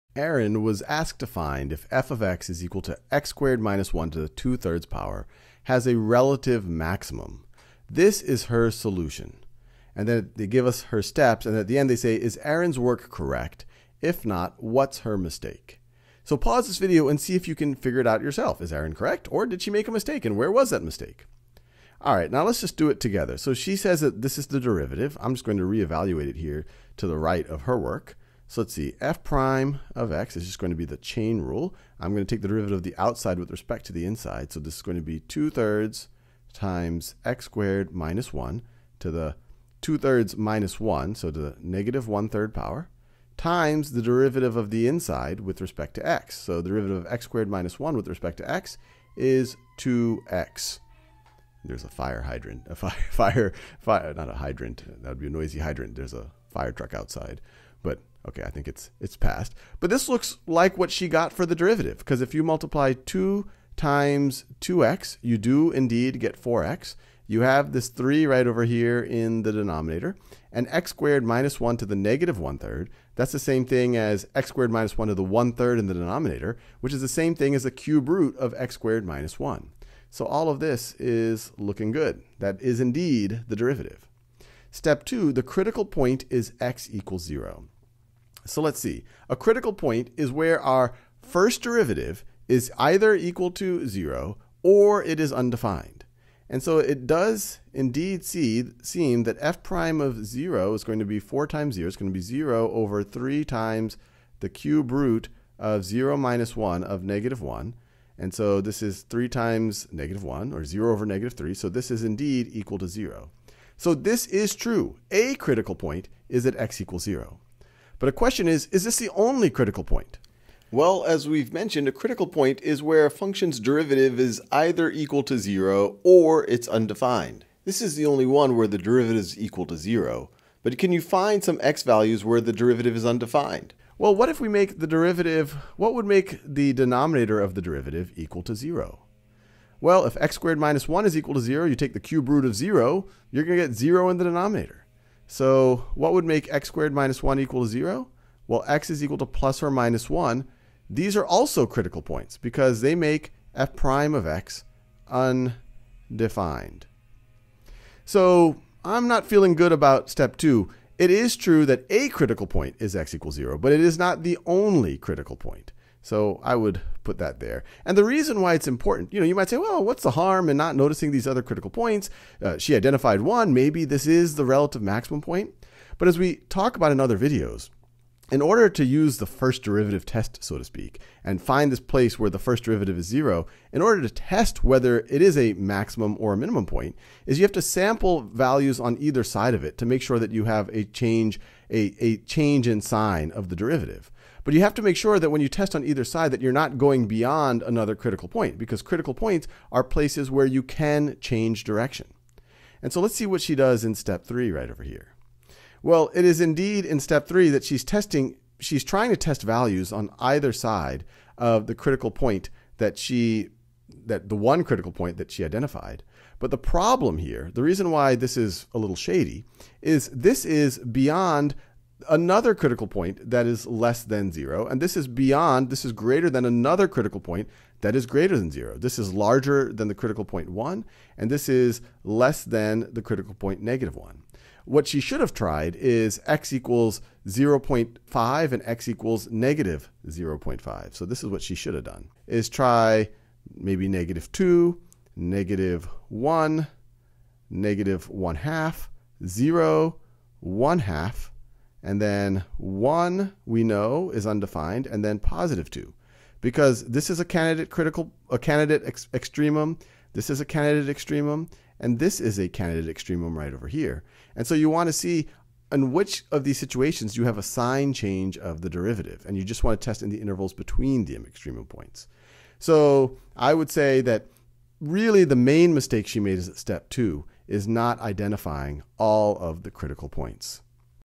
Analyzing mistakes when finding extrema example 2 | AP Calculus AB | Khan Academy
Analyzing the work of someone who tried to find extrema of a function, to see whether they made mistakes.

Category
Education